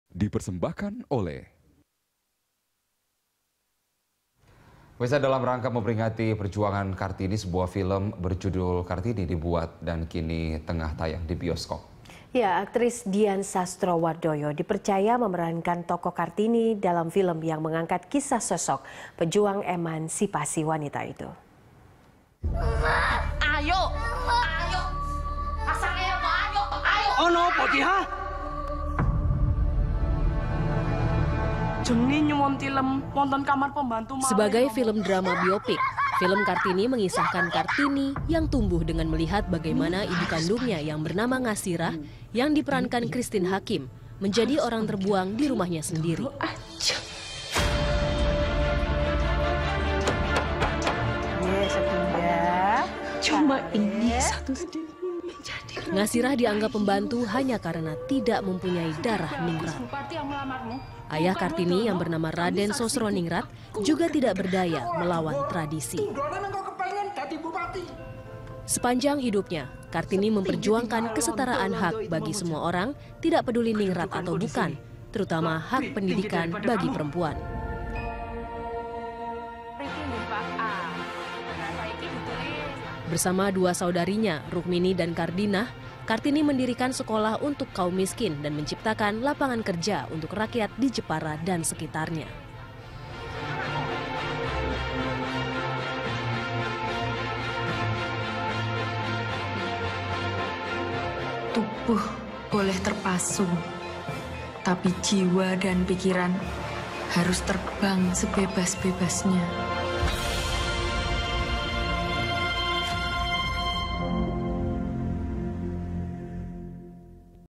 Film Kartini, Angkat Kisah Kartini Perjuangkan Emansipasi Wanita
Dalam rangka memperingati perjuangan Kartini, sebuah film berjudul Kartini dibuat. Aktris Dian Sastrowardoyo dipercaya memerankan tokoh Kartini dalam film yang mengangkat kisah sosok pejuang emansipasi itu.

Facebook.com/BeritaSatuTV
Youtube.com/BeritaSatu